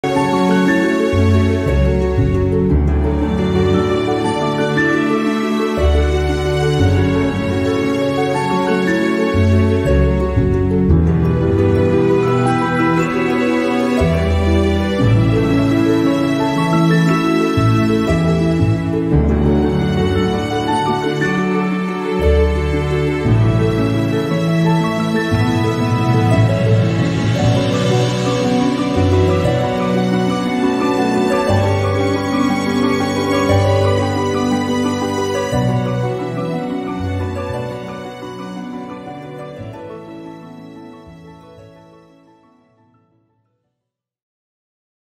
Book trailer for the book, The Magic in the Little Gemstone Kiosk
This drama romance book The Magic in the Little Gemstone Kiosk is inspired by a true story . This book is a stand alone book but also the sequel to Travesty of Justice book.